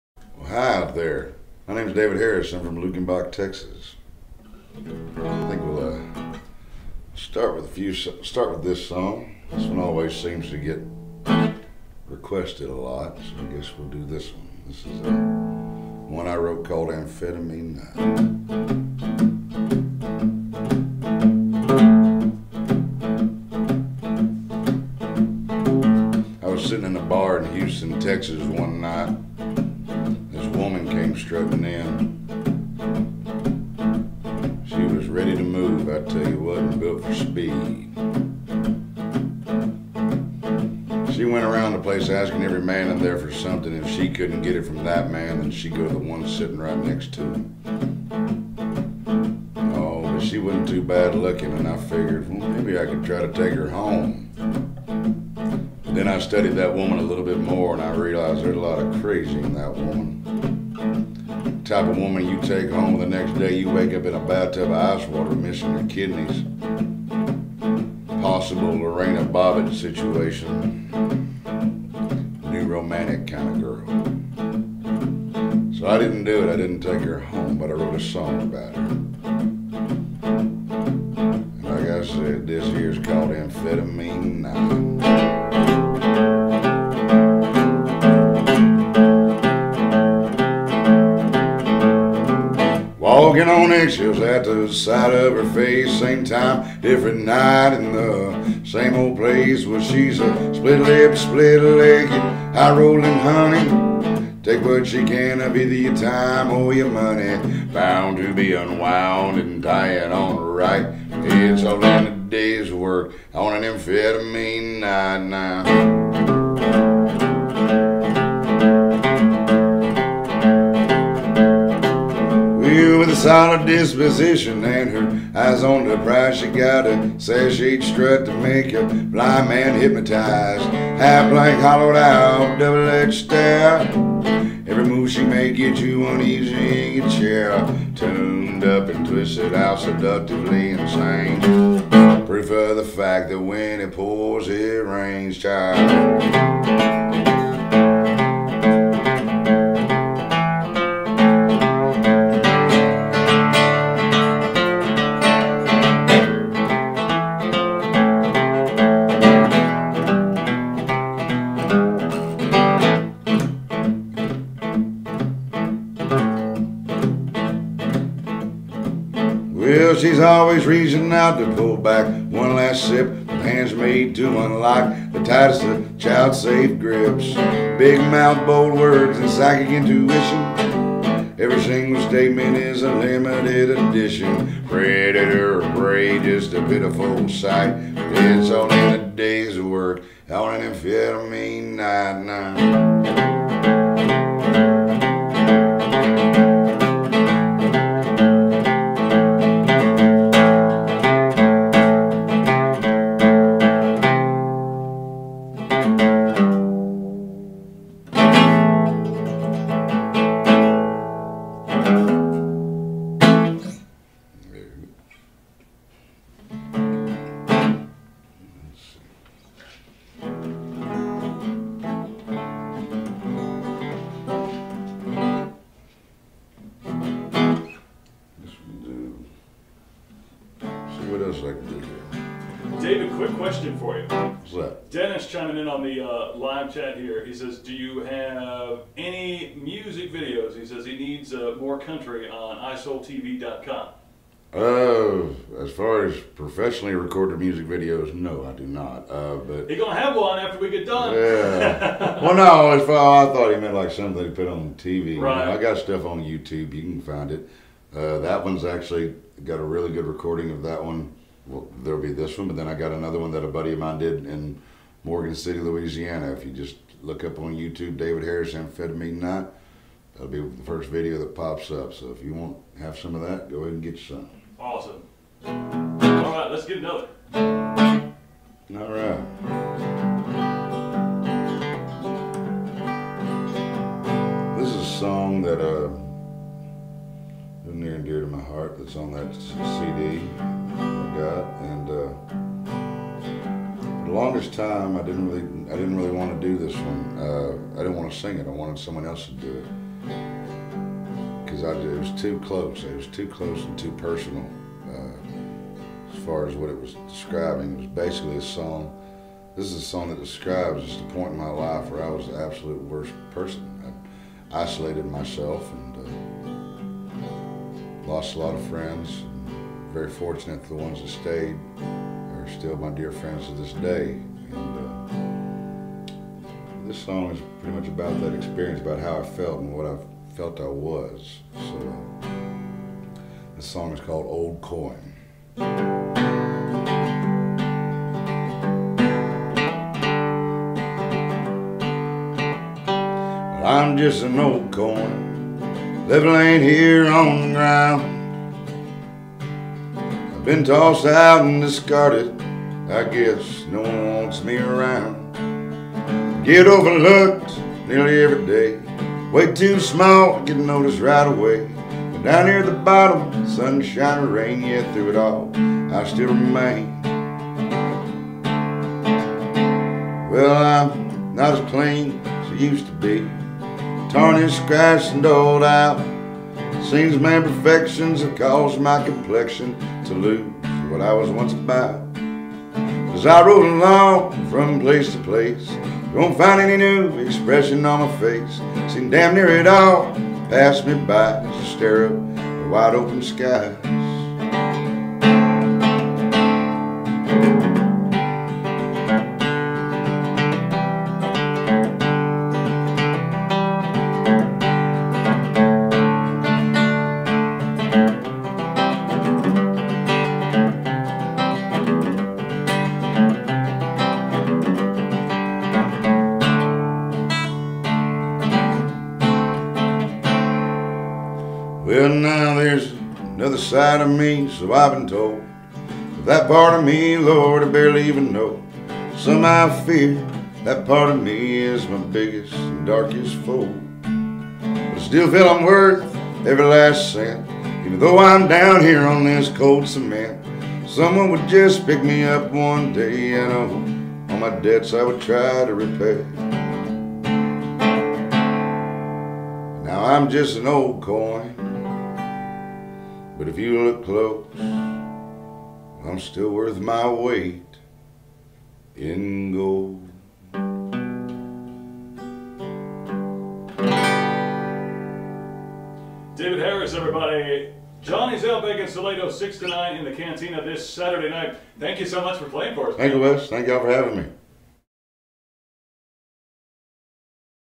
David Harris Performs in the US105 Green Room
Luckenbach  songwriter David Harris is becoming a regular performer at Johnny's Outback in Salado, and when you hear him, it'll become clear why.

With his soulful voice and relatable songwriting style, he's an artist you should definitely make it out to hear live!